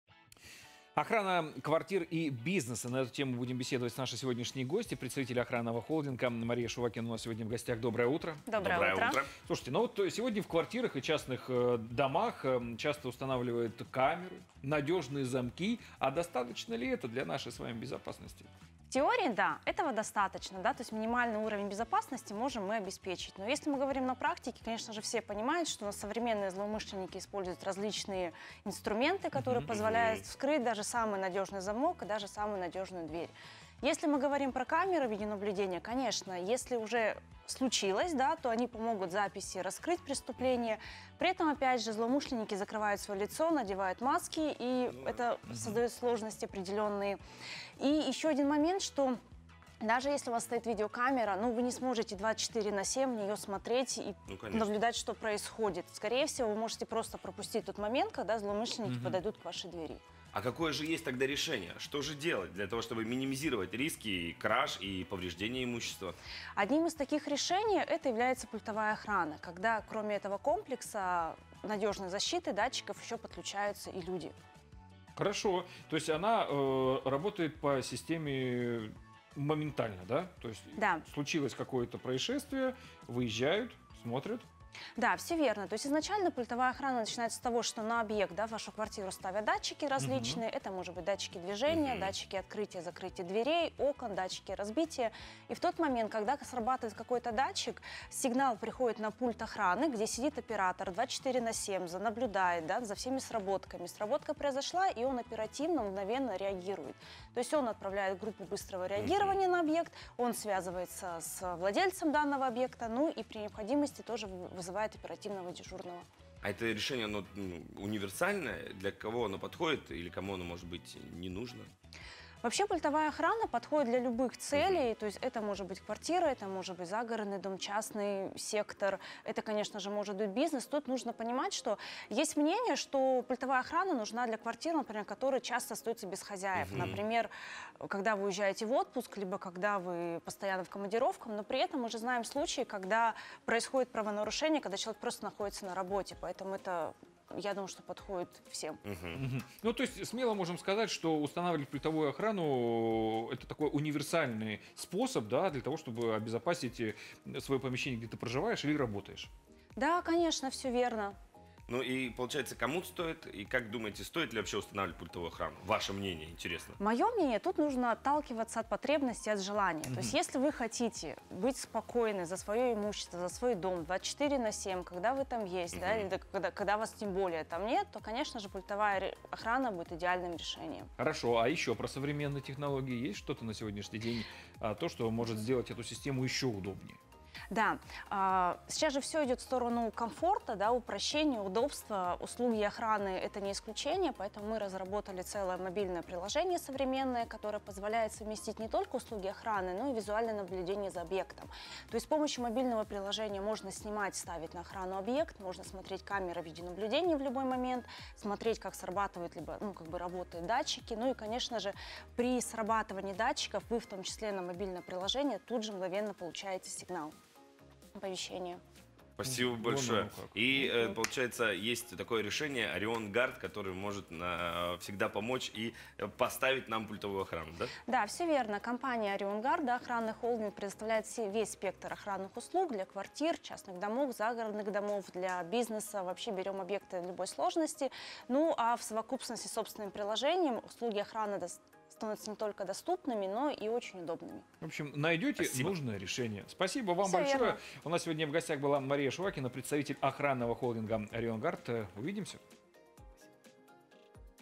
Охрана квартир и бизнеса
Как обеспечить максимальный уровень безопасности своей квартиры? Достаточно ли надежного замка и камеры, чтобы предотвратить кражу? И какие современные способы защиты имущества предлагают специалисты? Поговорили с представительницей охранного холдинга Марией Шувакиной.